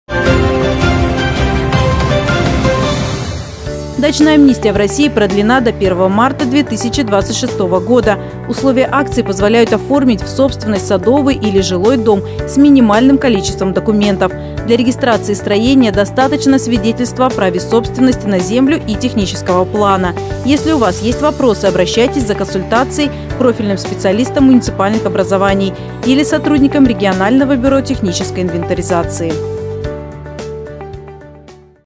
Дачная амнистия
«Дачная амнистия» в России продлена до 1 марта 2026 года. Условия акции позволяют оформить в собственность садовый или жилой дом с минимальным количеством документов.